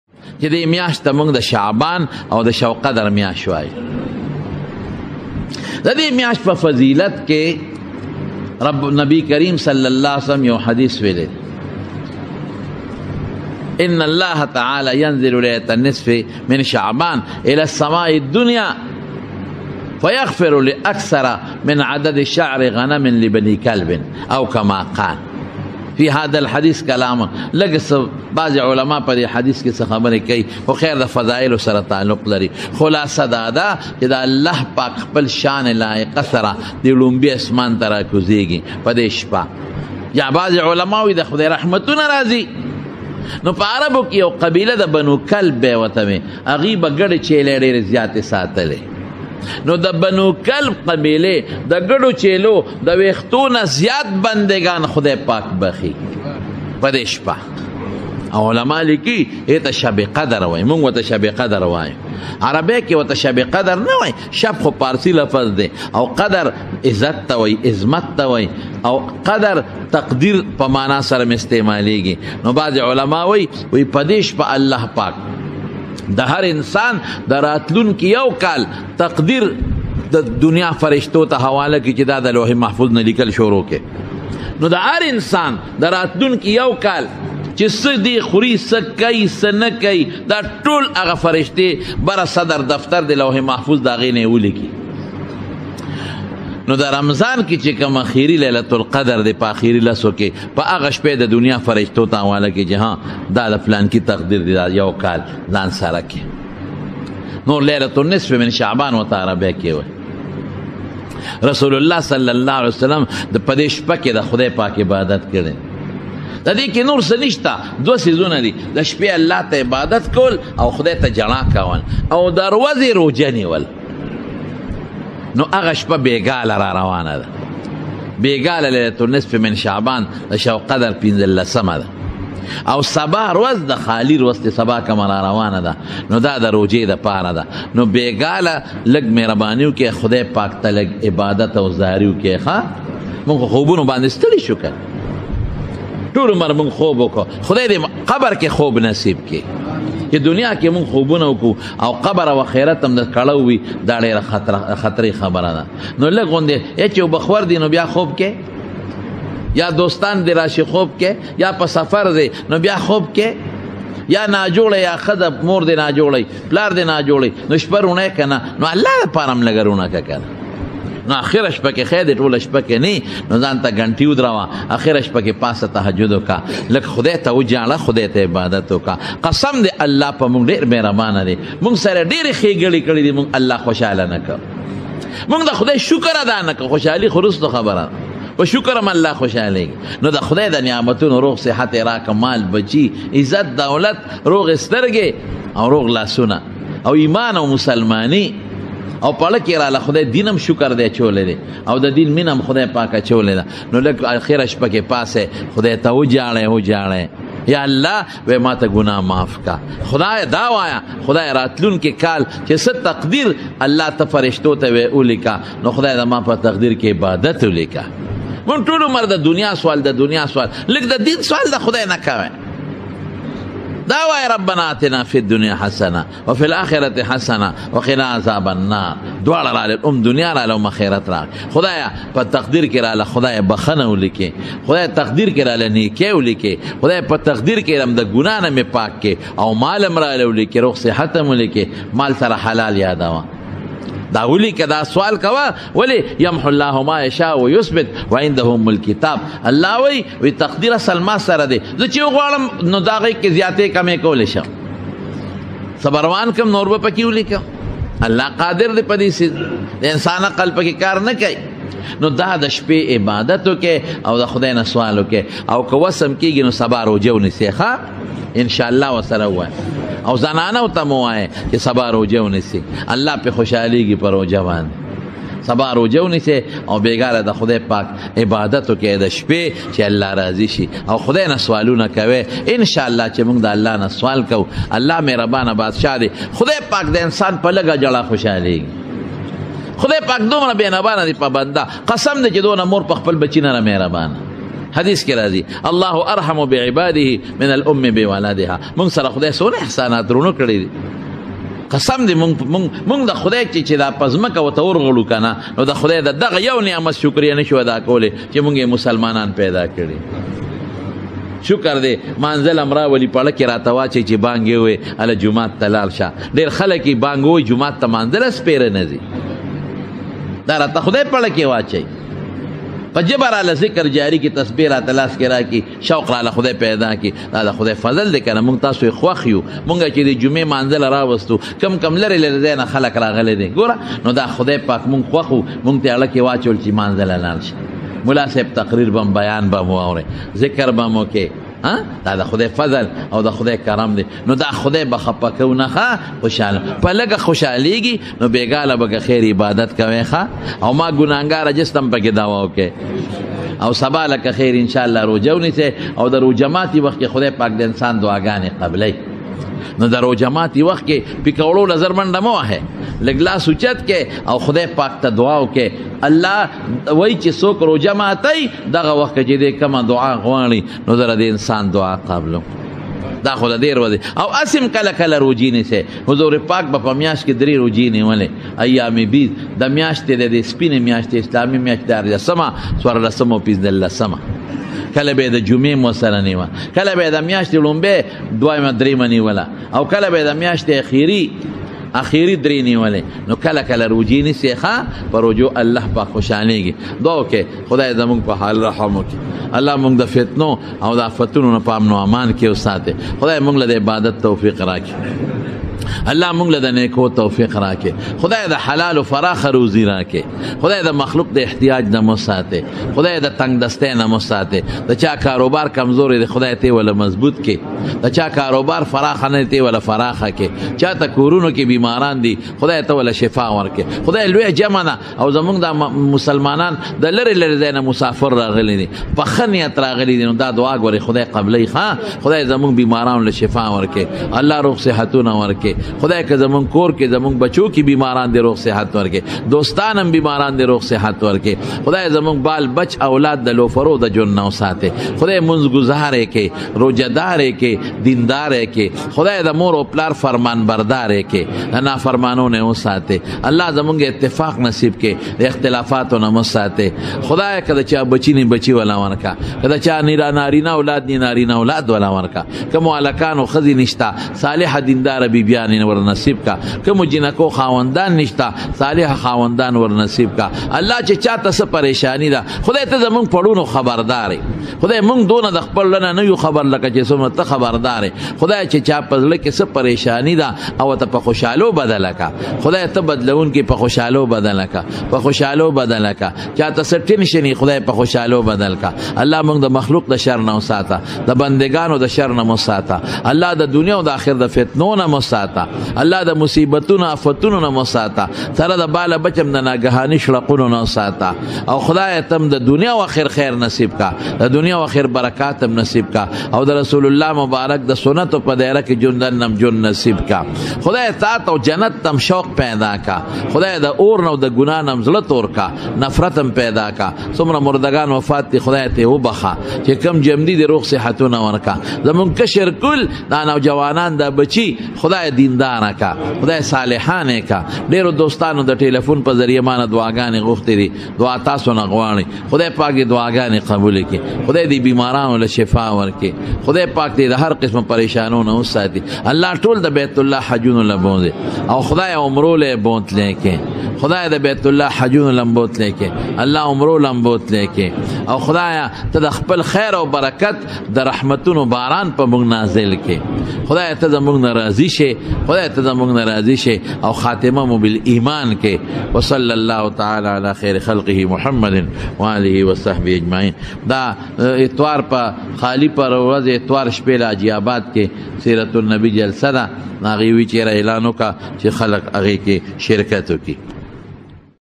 Shaikh UL Hadees Molana Muhammad Idrees Sahib II Pashto Bayan II Shabi Baraat II Tareekh - E - Islam
This My Channel is Tareekh - E - Islam Islamic Channel This Good Naat ,story Islamic and Butifull Ayaat Others